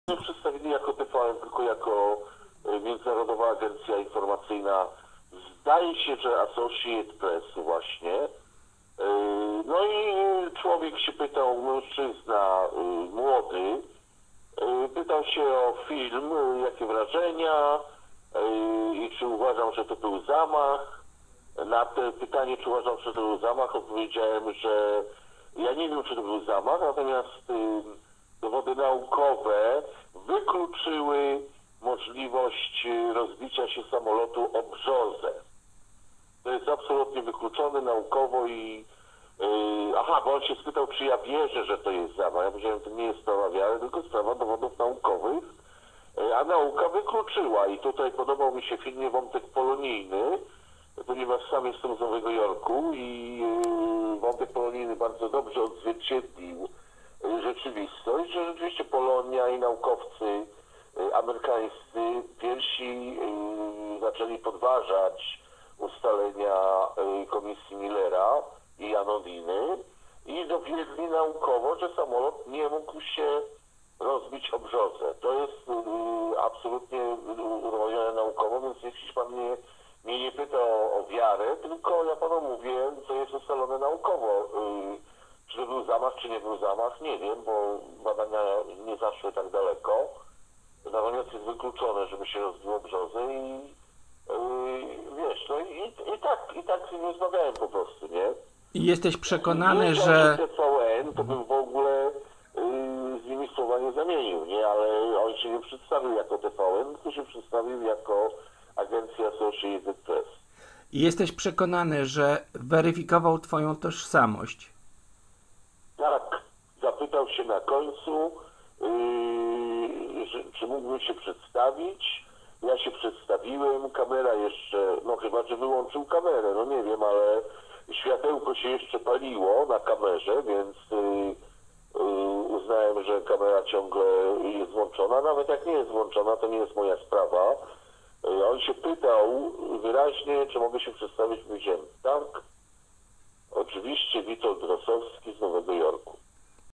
TVN ciągle kłamie !!!
Każdy kto był na filmie „Smoleńsk” łatwo znajdzie pierwowzory osób czy zachowań, które jednoznacznie mogą się kojarzyć ze stacją telewizyjną z Wietrniczej.

Po 10 kwietnia masowo byliśmy karmieni informacjami typu „kłótnia przed odlotem”, „jak nie wyląduję to nas zabije”, „pijany pilot” i cała masa innych.

Obraz Krauzego przypomniał nam te manipulacje/kłamstwa, za które żaden dziennikarz nigdy dotąd nie przeprosił.

Dziś, jak się okazuje, TVN ciągle „na tym samym kursie i ścieżce”.

Najwyraźniej obecnym na premierze filmu wysłannikom tej zacnej stacji telewizyjnej zabrakło wypowiedzi istotnych osób, więc postanowili wyłapywać z tłumu wychodzących, by przedstawiać je, jako te, które mogą mieć coś do powiedzenia.

Nie dziwi, że większość nie chciała wypowiadać się do kamery, bo a rusz podpiszą cię później jako …np. Artur Wosztyl.

Tak też się stało w przypadku Witolda Rosowskiego, prezesa OT „Solidarni 2010”NY/NJ.

Na swoim FB Artur Wosztyl napisał:

Od znajomych dotarły do mnie informacje, że wypowiadałem się na temat filmu "Smoleńsk" w reżyserii Pana Antoniego Krauze dla telewizji TVN zaraz po jego uroczystej prapremierze. Zdziwiony byłem tymi sugestiami i przy pomocy mi bliskich znalazłem ten materiał. Jestem zaskoczony brakiem profesjonalizmu mediów, które kształtują opinię publiczną. Jestem zażenowany tym, że podaje się Polakom nieprawdę. Właśnie tak wygląda manipulacja i próba przedstawiania rzeczy przez media, które sympatyzują z pewnymi siłami politycznymi, a które odrzucają kilka groszy do tego,aby dyskredytować sam film, który poruszył mnie. Polecam uwadze ten krótki film i jednocześnie mam nadzieję, że telewizja TVN naprawi swój błąd, albo celowe działanie.

Oto, co m. in. powiedział nam Witold Rosowski:

Nie przedstawili się jako TVN tylko jako międzynarodowa agencja informacyjna (chyba Associated Press). Młody mężczyzna pytał się o film, o wrażenia, czy to był zamach. (...)Na pytanie czy to był zamach, odpowiedziałem, że nie wiem ale dowody naukowe wykluczyły możliwość rozbicia się samolotu o brzozę. Spytał się czy ja wierzę, że to był zamach? Odpowiedziałem, że to nie kwestia wiary ale dowodów naukowych. (...)Gdybym wiedział, że to TVN, to bym słowa z nimi nie zamienił. (...)Na końcu dziennikarz zapytał czy mogę się przedstawić. Powiedziałem: tak, nazywam się Witold Rosowski z Nowego Jorku.